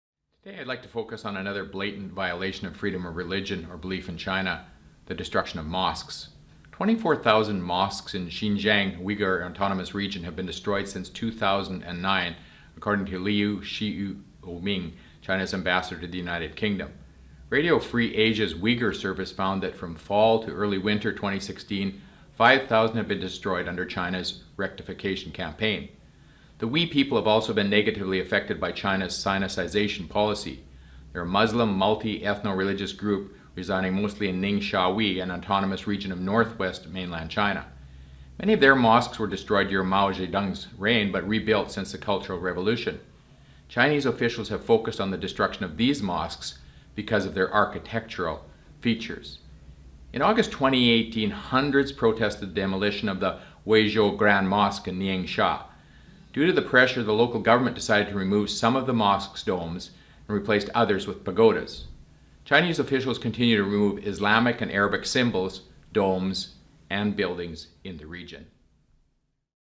Religious Persecution in China - Ep. 2: The Destruction of Mosques
MP David Anderson provides an update on Freedom of Religion or Belief in China - this time addressing the continued destruction of mosques.

Click here to access recent news on Freedom of Religion or Belief in China, including MP Anderson's latest reports